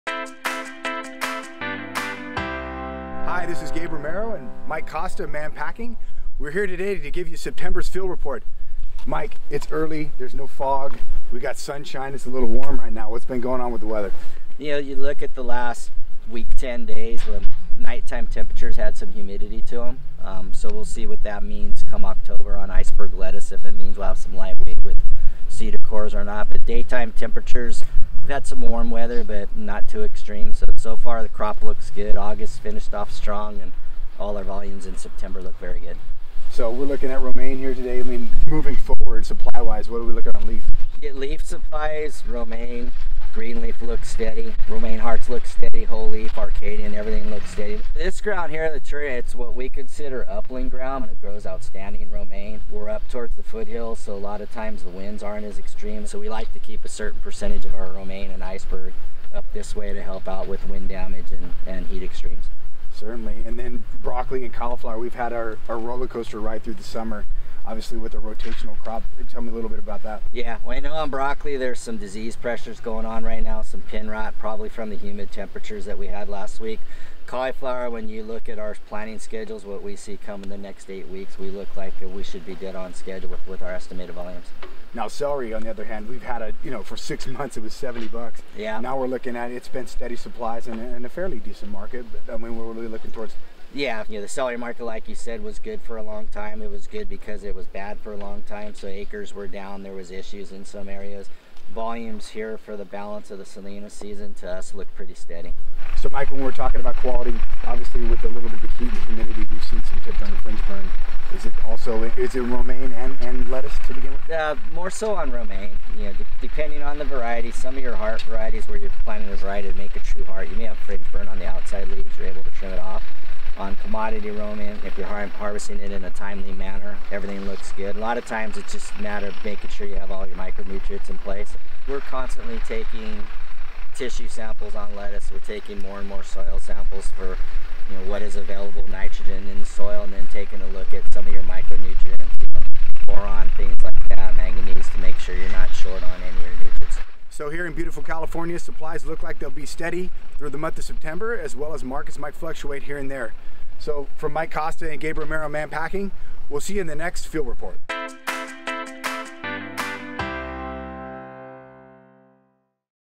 Mann’s Field Report September 2019 - Romaine
From outside of Salinas, here is an update for September 2019 with Gabe Romero and Mike Costa from the field on romaine, broccoli, cauliflower and celery.